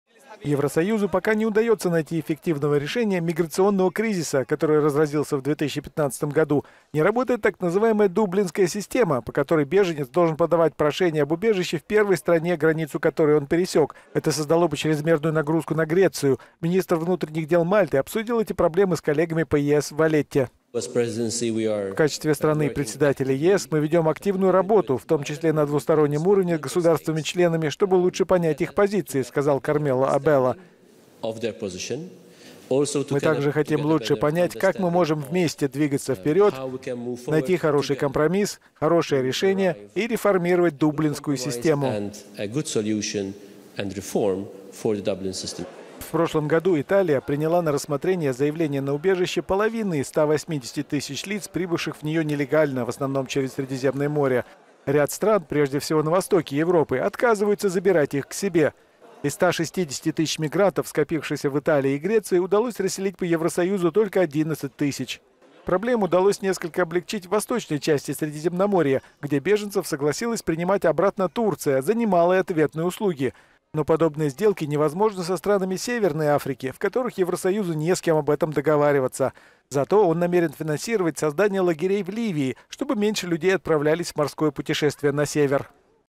У ЕС огромная проблема - "Дублинская система" не работает!
Евросоюзу пока не удаётся найти эффективного решения миграционного кризиса, который разразился с наибольшей силой в 2015 году. Не работает так называемая “Дублинская система”, по которой беженец должен подавать прошение об убежище в первой стране, границу которой он пересёк. Это создало бы чрезмерную нагрузку на Грецию.
Министр внутренних дел Мальты Кармело Абела обсудил эти проблемы на встрече с коллегами по ЕС в Валетте.